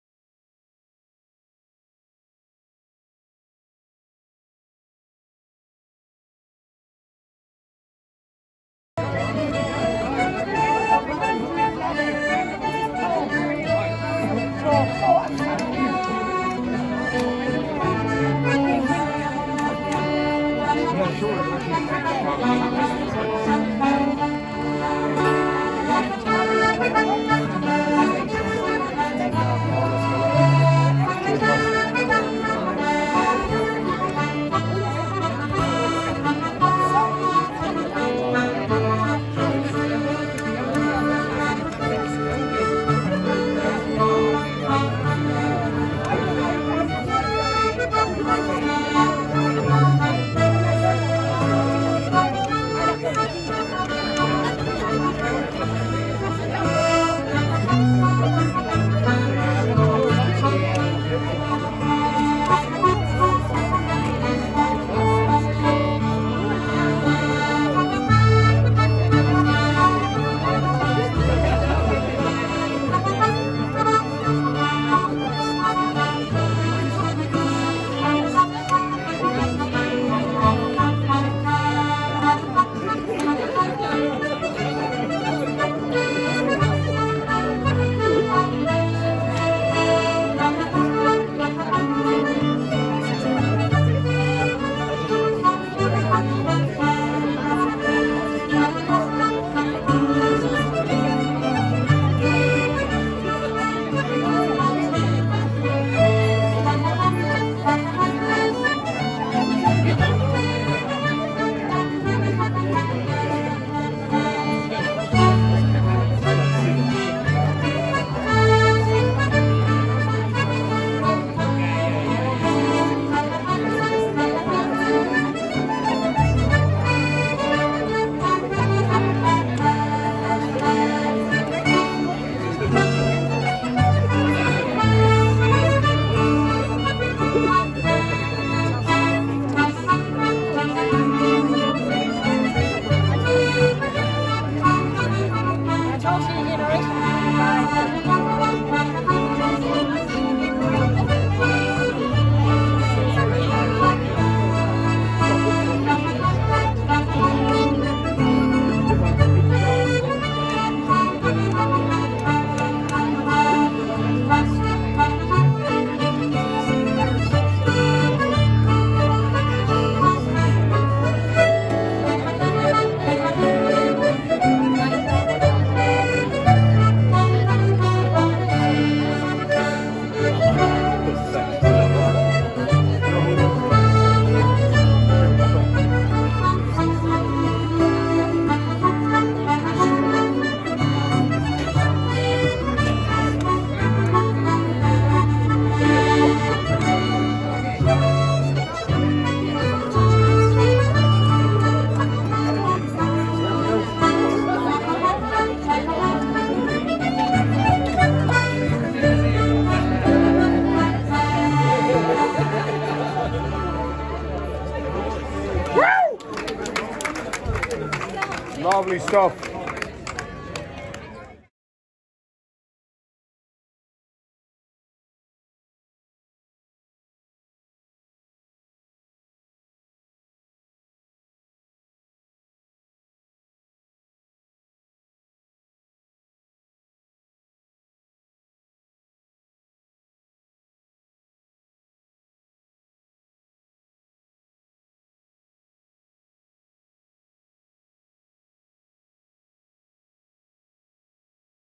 Time Zones Exhibition Launch tunes with Graham Dunne, Claire Egan and Eoin O'Neill.
Graham Dunne, Claire Egan and Eoin O'Neill playing a few tunes at the launch of Time Zones, an exhibition of paintings and drawings by Terry Smith at the Art in Mind Gallery in Galway Ireland.

For over twenty years, film goers who made their way to the Claddagh Palace would have been entertained by a busker, strumming a guitar and singing his heart. The Claddagh Palace was an old style picture palace and the busker was Terry Smith.

What many may not realize is that in later life Terry returned to education to obtain a degree in fine arts. This collection is part of the immense body of work he created for the last 20 years of his life. Join us in celebration of the life and works of Terry Smith.